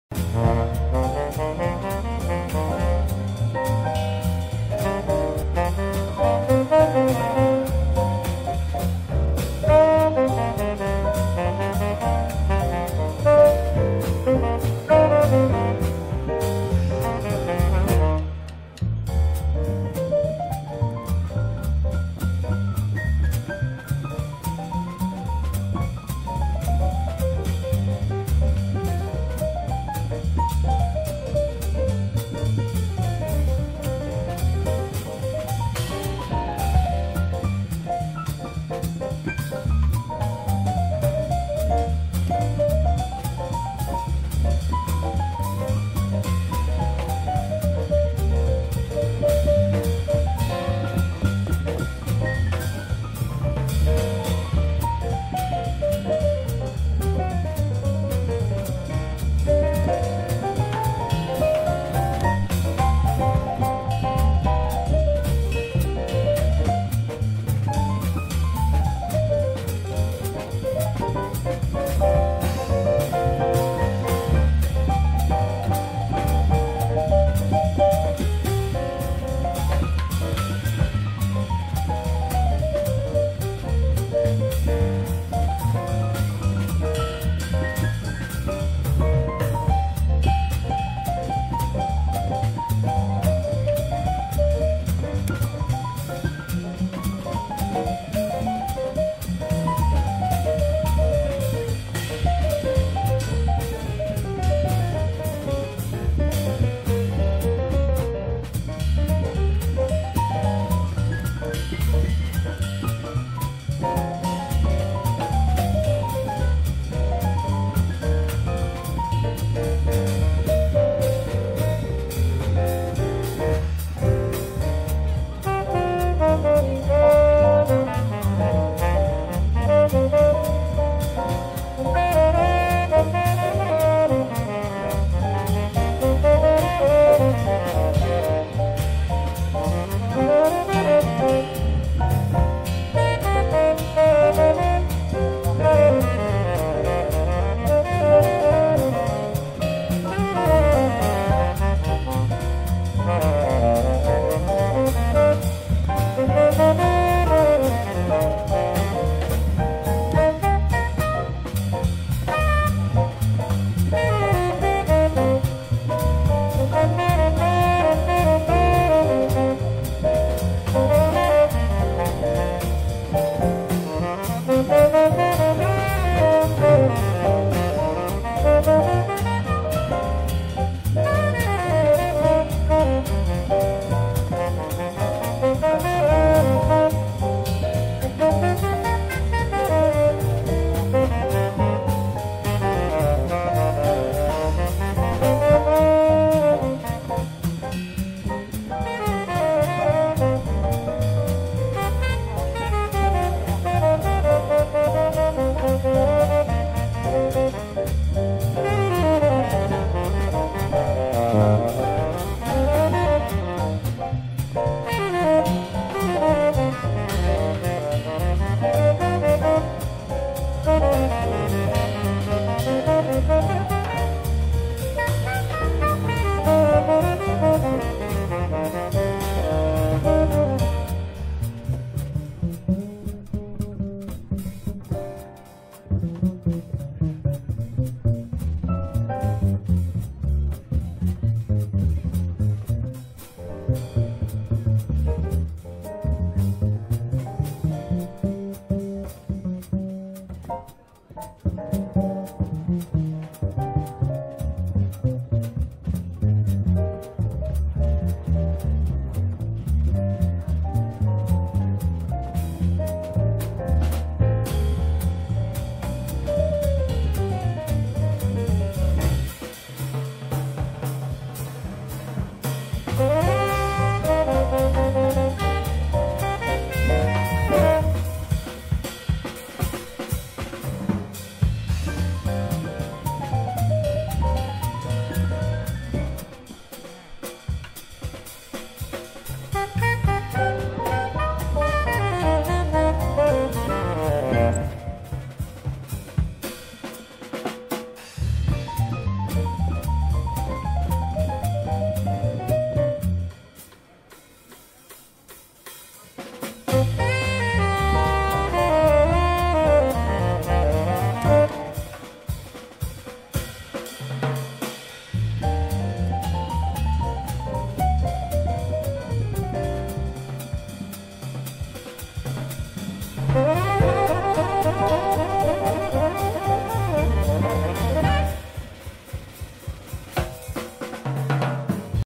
Jazz in Algarve - Tavares Jazz Quartet
Jazz in Algarve happens every Sunday at Quinta do Moinho Restaurant, in Loule, Algarve. The Tavares Jazz Quartet led by pianist Fernando Tavares brings in fabulous professional jazz musicians and a great repertoire which includes jazz, bossas, sambas,  latin jazz and pop standards. Available to perform for any type of event, weddings, corporate and private parties in Algarve, Alentejo and Lisbon.